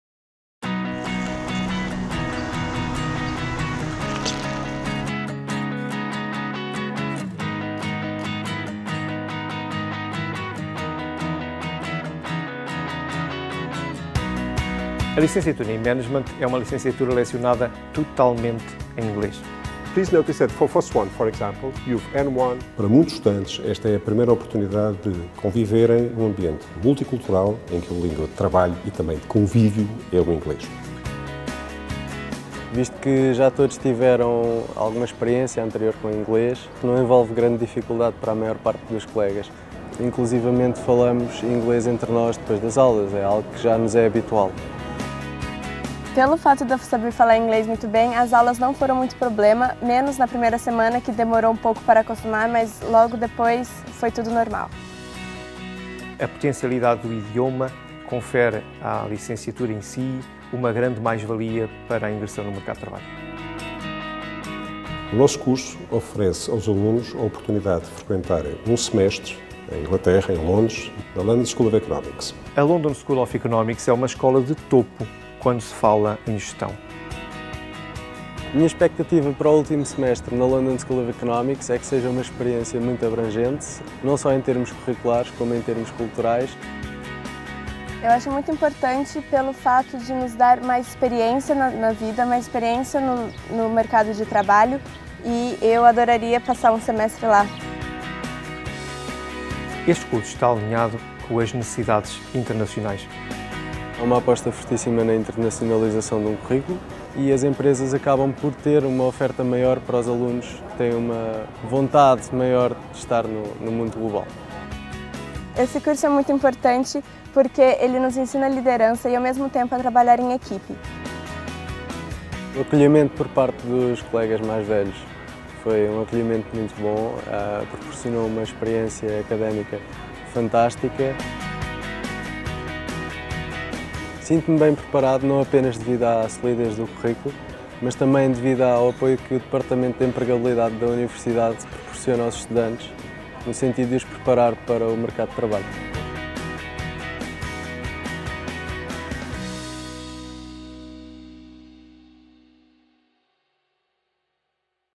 Licenciatura Management | Universidade Europeia
A licenciatura em Management & Business Administration em associação com LSE Custom Programmes na London School of Economics and Political Science, possibilita a frequência do último semestre nesta escola de referência a nível mundial. Sabe mais